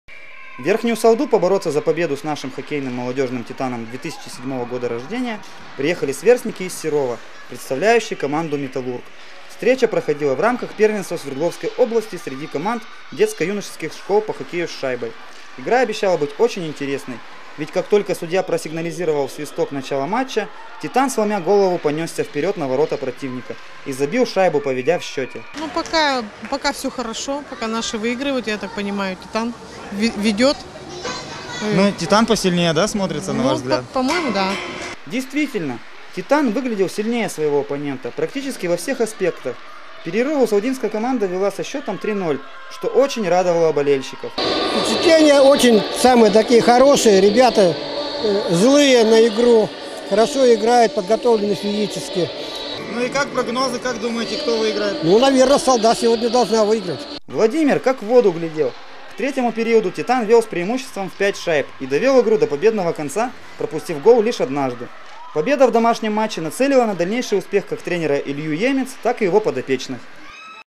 «Титан» против серовского «Металлурга»
Спортсмены из секции хоккея открыли новый сезон. На ледовой арене  «Старта» в минувшую субботу прошли первые игры чемпионата Свердловской области по хоккею.